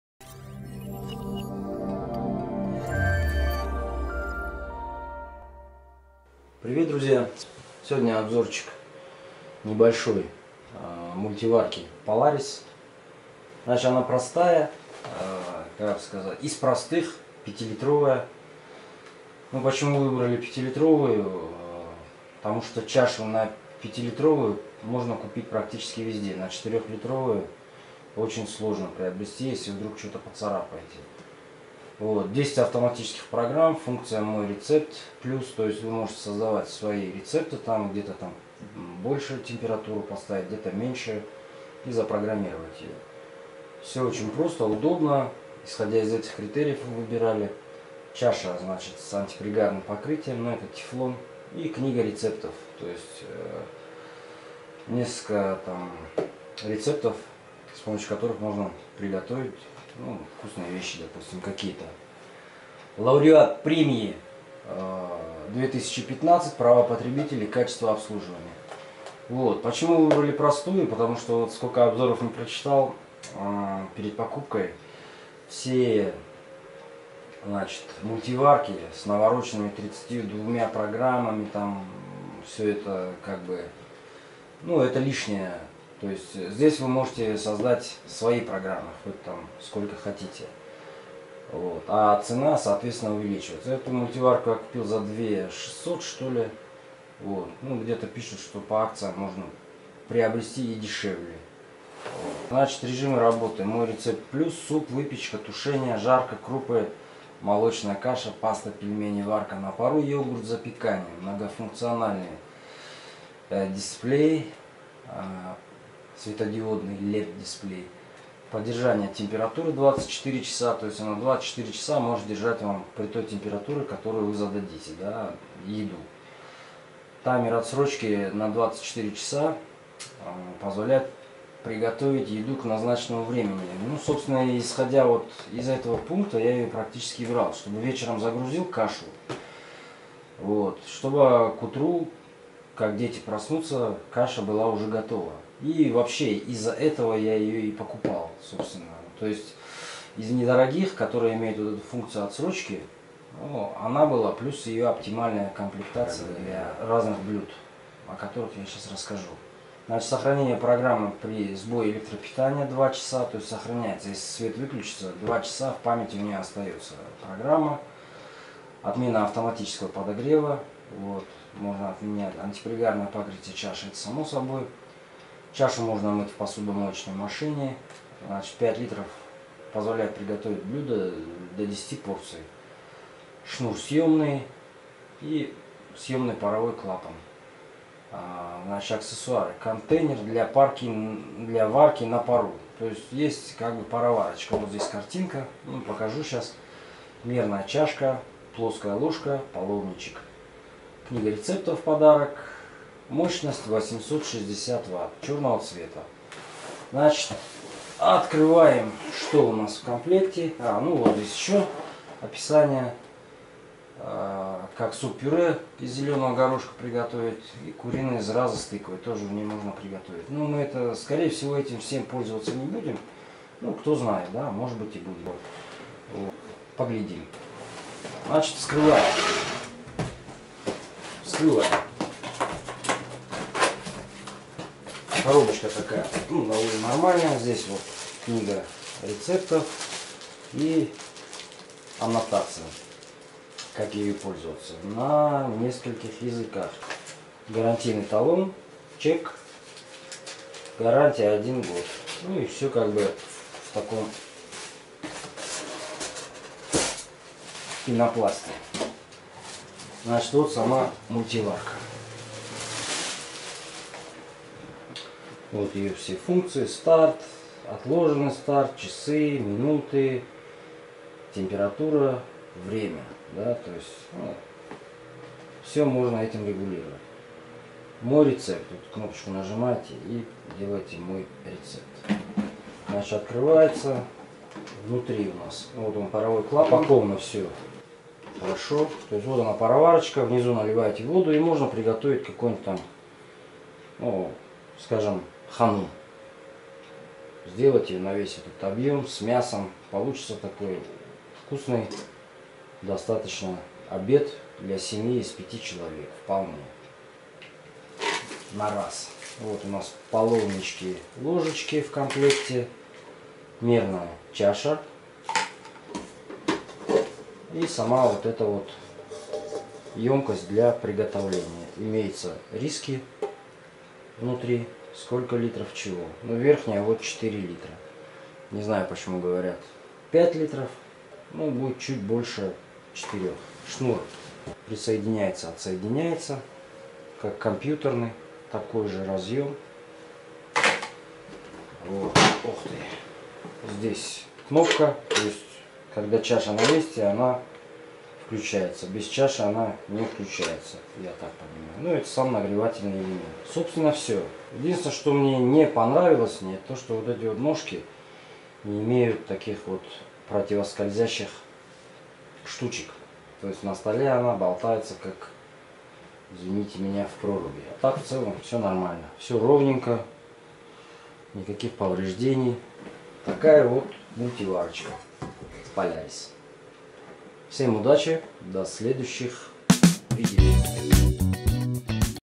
Мультиварка POLARIS PMC 0548AD
Обзор и комплектация мультиварки Polaris pmc 0548adm - простая, от того надежная и вполне себе бюджетная. С пароваркой и двумя половничками, один из которых шершавый.